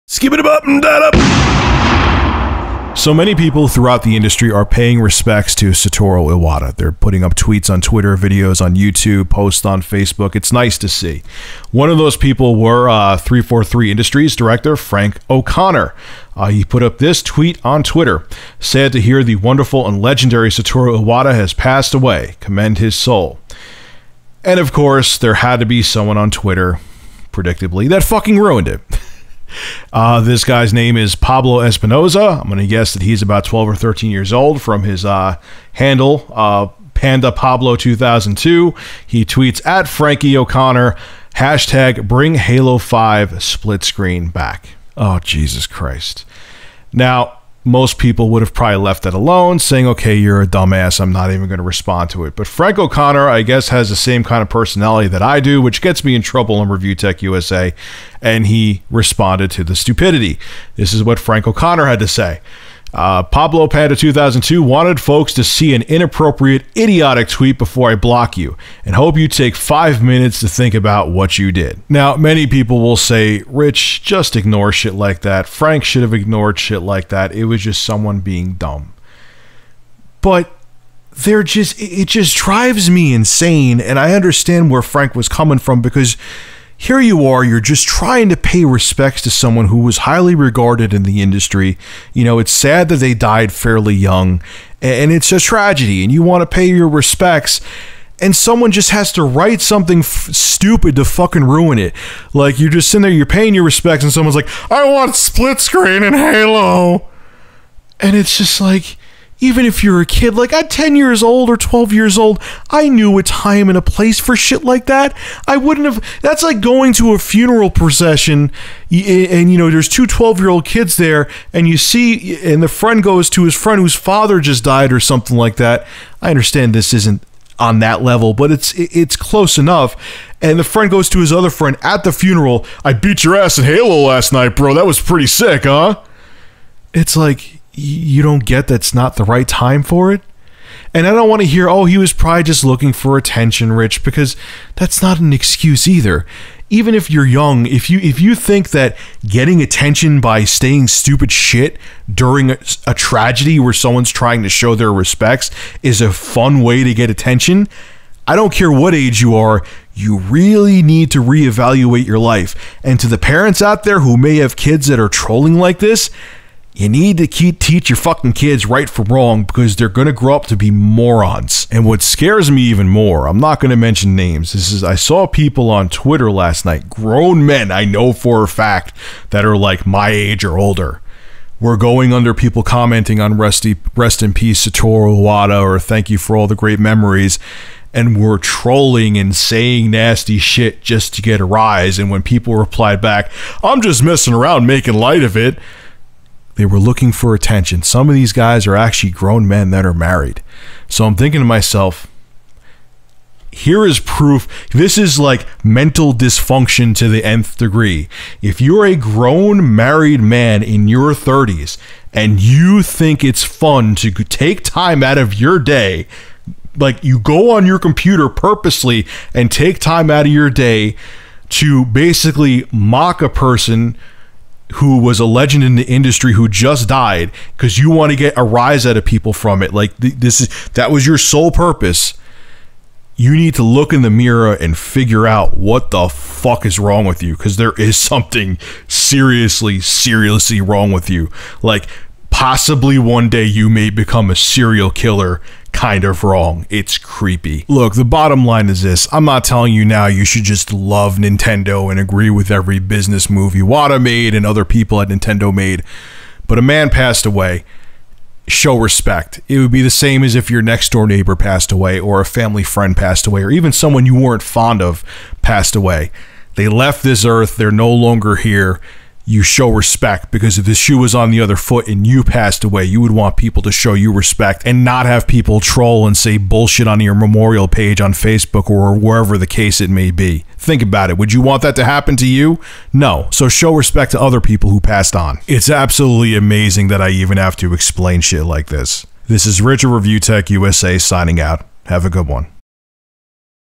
343's Frank O'Connor Rips A Twitter Troll A New One
Are you a YouTube content creator? Click the link to apply for a Curse Entertainment Partnership: